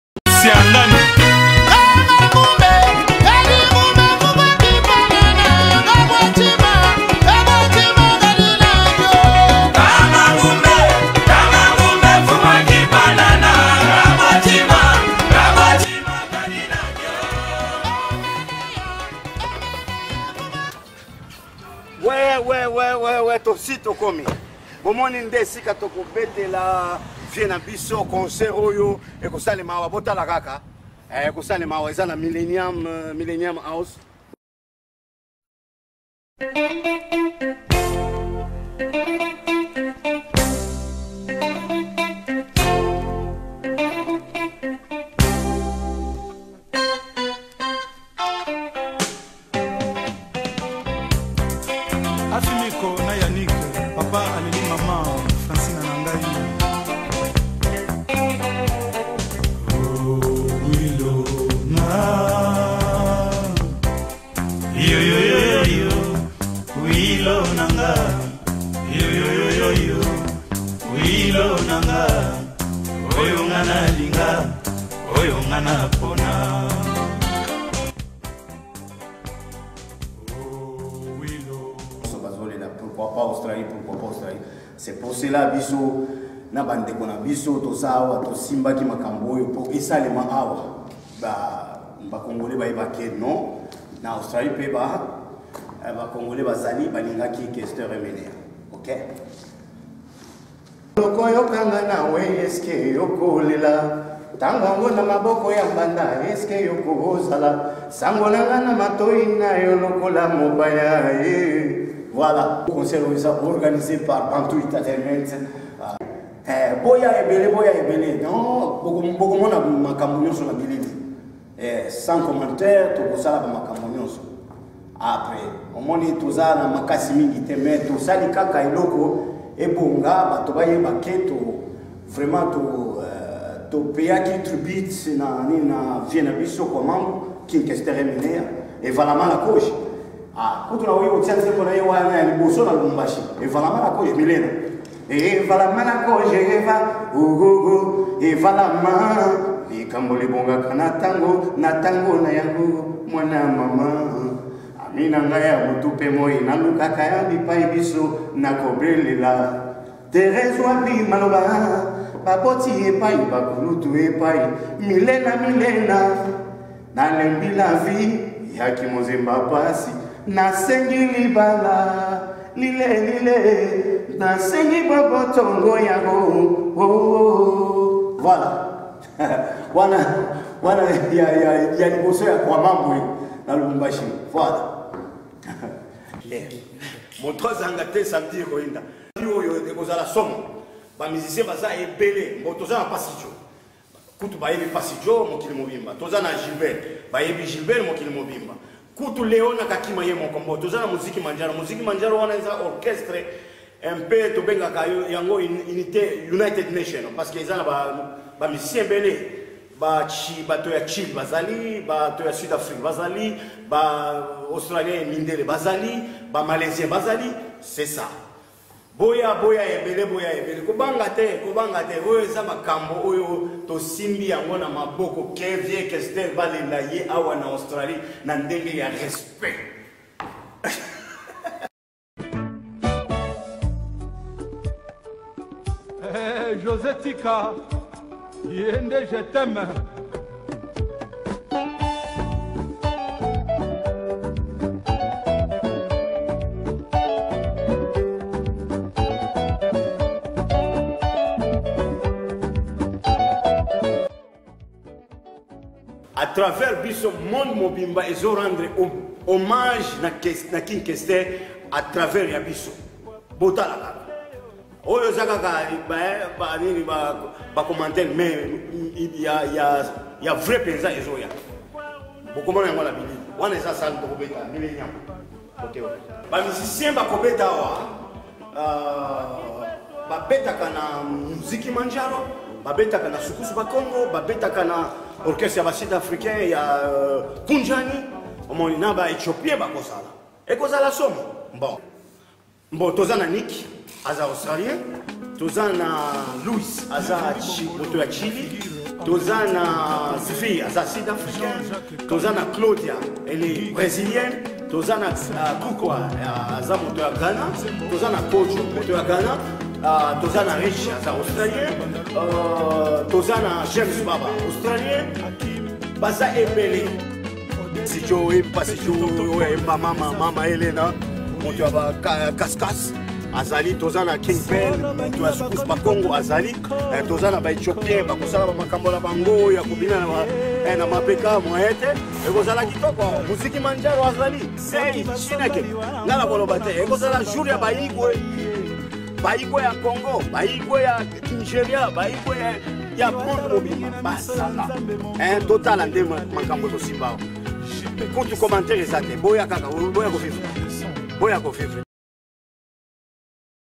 Annonce de la grande soirée d’hommage à King Kester Emeneya à Melbourne /Australie
En collaboration avec les communautés africaines de Melbourne, Bantu Entertainment Production organise ce samedi 30 août 2014 une soirée d’hommage à King Kester Emeneya à Melbourne /Australie

Lieu : Millenium House
         296 Nicholson Street Footscray
Heure : 20 h
Musiciens invités: Leona Kakima /Passi Jo/ King Bell
(tous serons accompagnés par le groupe Musiki Manjaro)